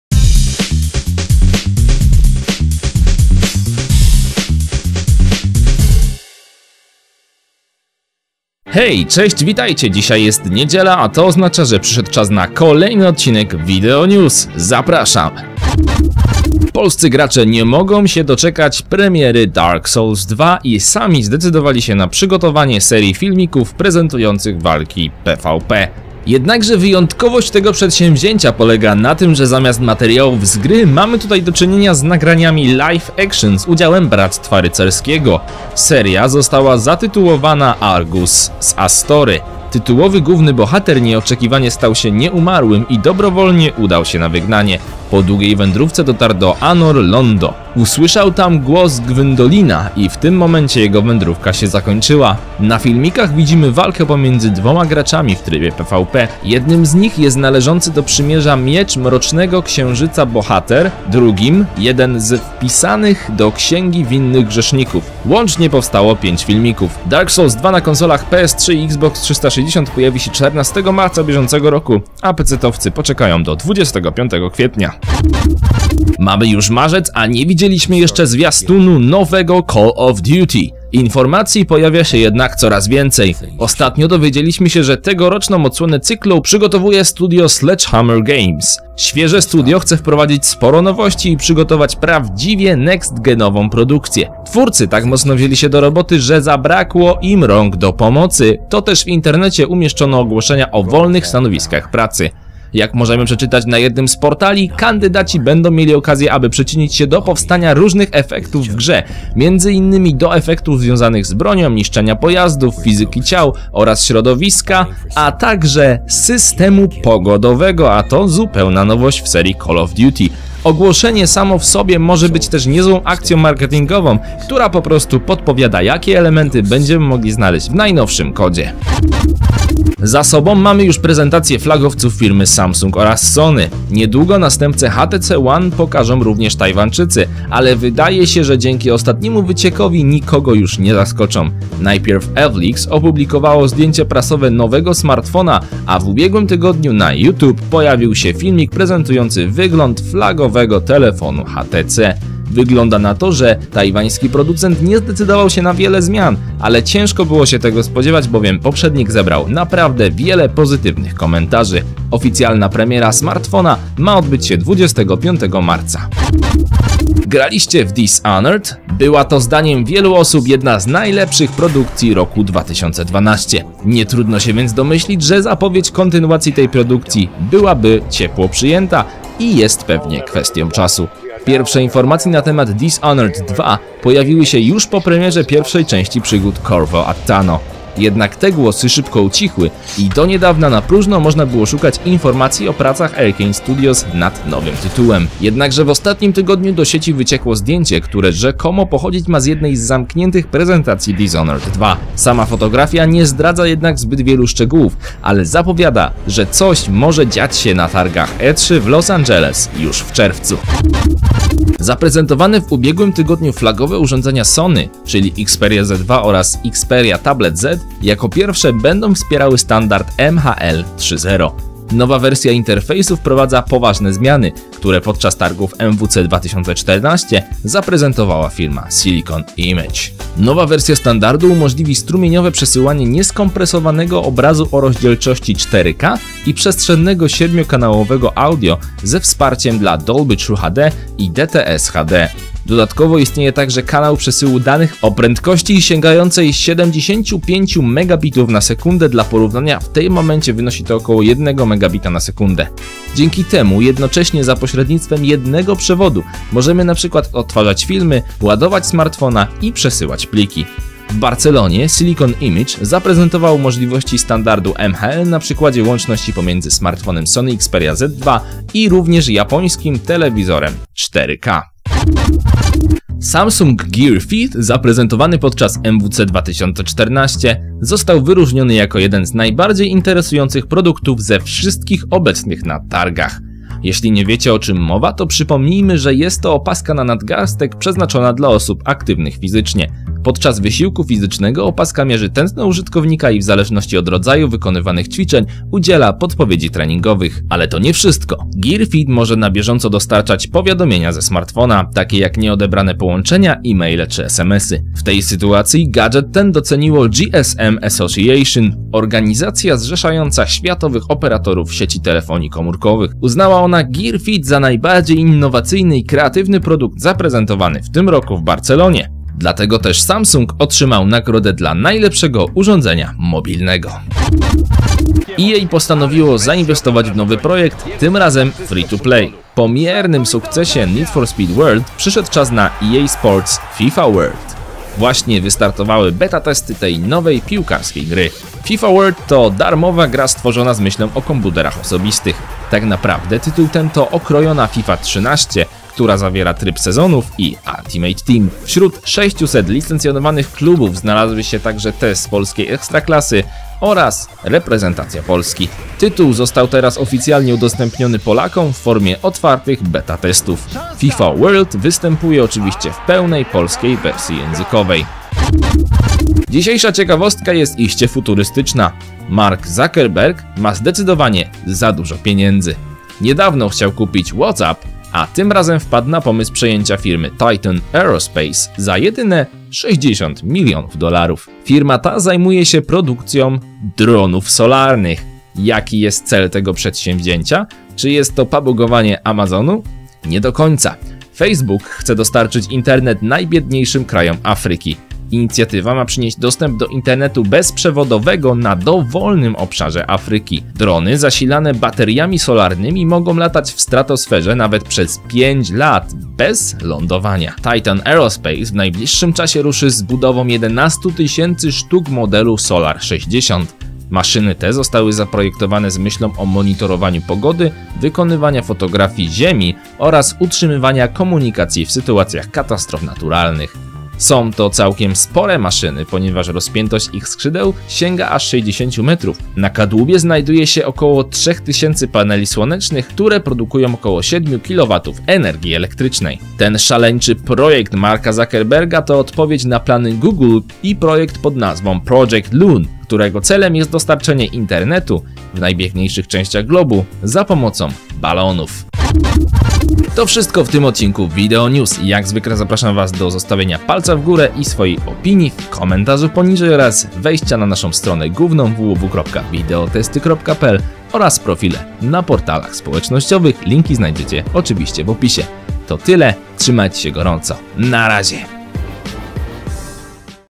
VideoNews 19# - zobacz jak wygląda następca HTC One...
W kolejnym odcinku VideoNewsów znajdziecie informacji nt. premier gier Dark Souls II, Call of Duty i Dishonored 2. Zobaczycie również "przecieki" nt. wyglądu następcy smartfona HTC One. Zapraszam do oglądania i komentowania...

Redakcja Portalu
VideoTesty.pl